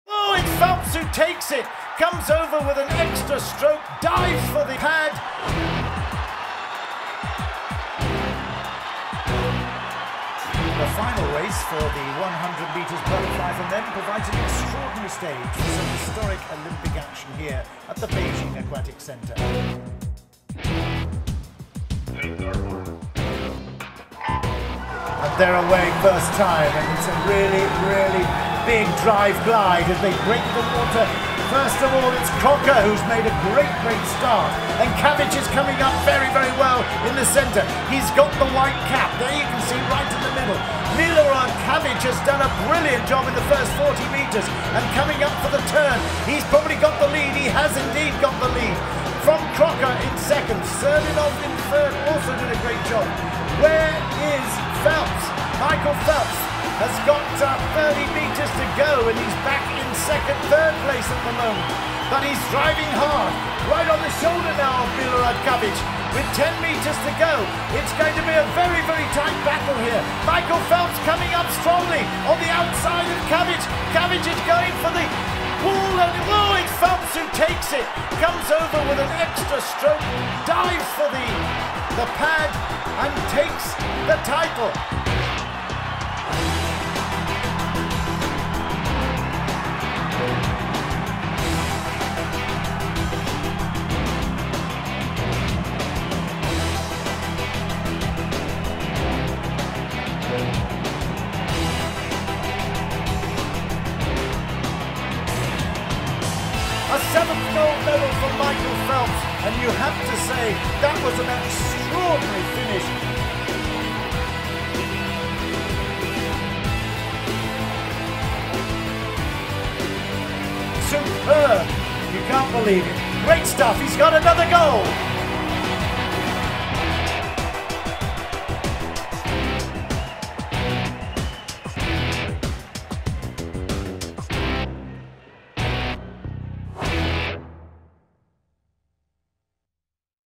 Closest swimming finish EVER! Ft. Michael Phelps ⏱️
In the thrilling finale of the 100m butterfly event at the Beijing 2008 Olympics, Michael Phelps left the world awestruck with an astonishingly close finish. As the race reached its climax, Phelps engaged in an intense battle with Serbian swimmer Milorad Čavić. Stroke by stroke, they surged toward the wall, both determined to touch it first. With hearts pounding and spectators holding their breath, the outcome remained uncertain until the very last moment.

In the end, Phelps managed to stretch his arm 4.7mm further forward and take the victory by just one hundredth of a second. This thrilling race went down in swimming history as one of the closest finishes and cemented Michael Phelps' legendary status in the world of competitive swimming.